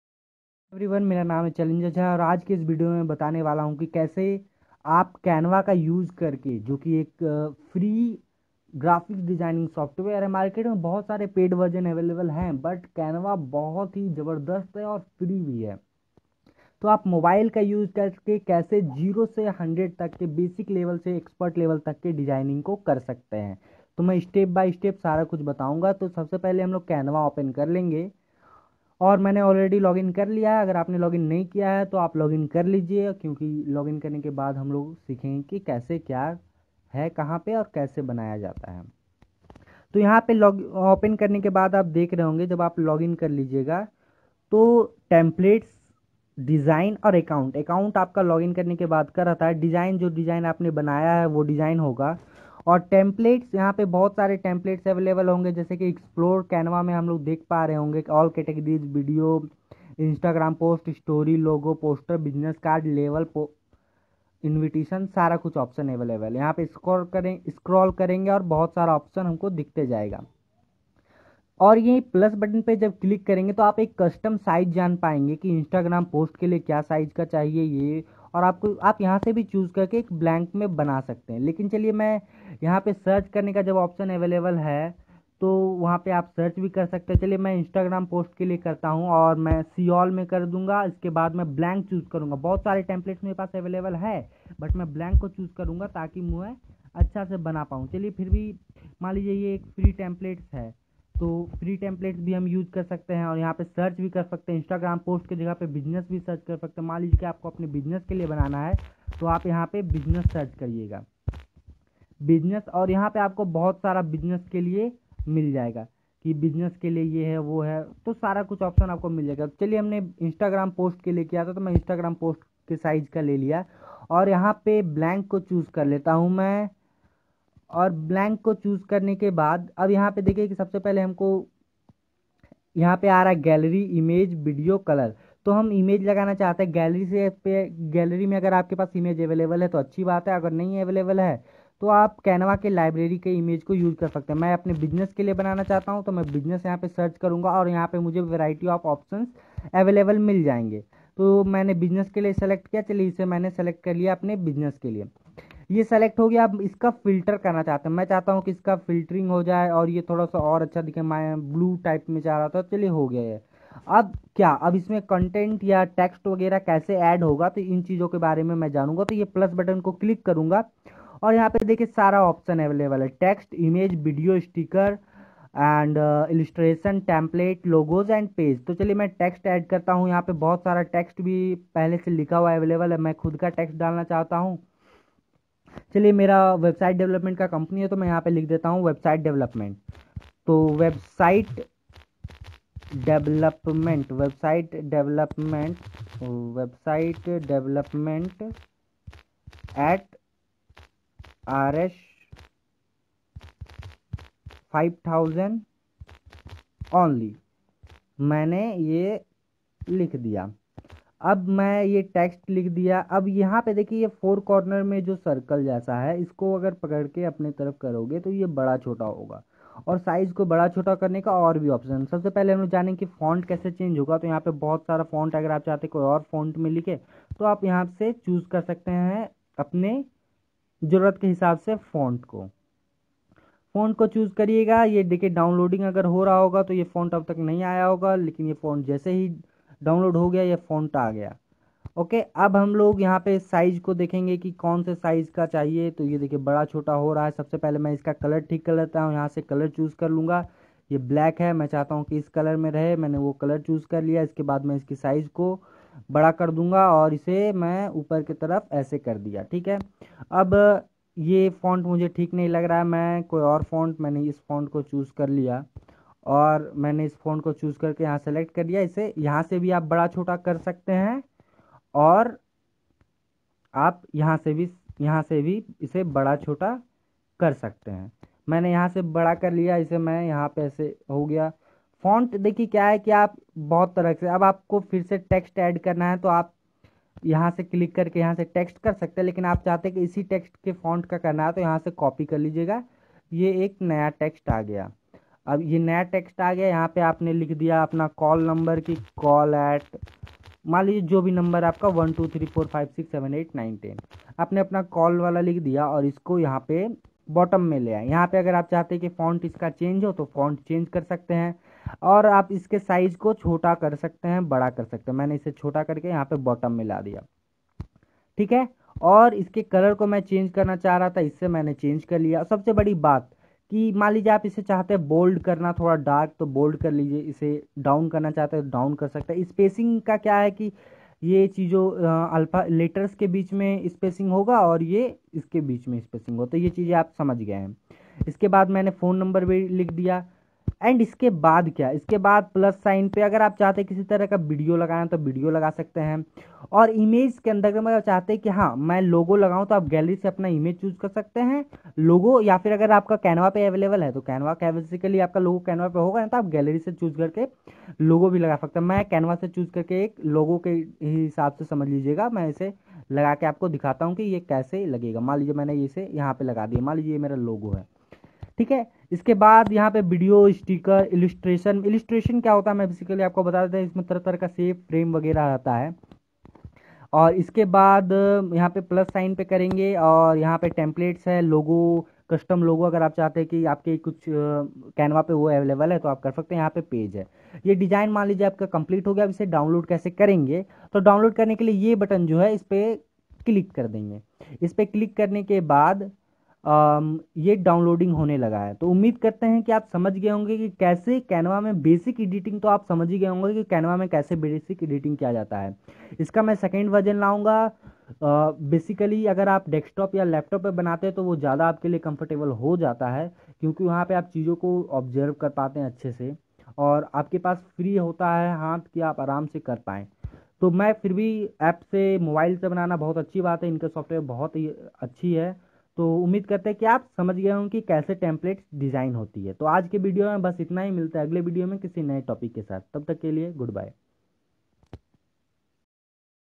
Free Graphics Design Software 2020 | Beginner's Guide to Canva | Challenger Jha
This video is about free graphics designing mobile application.
This software is free and has awesome features.

Watch this video till end to learn how to use canva.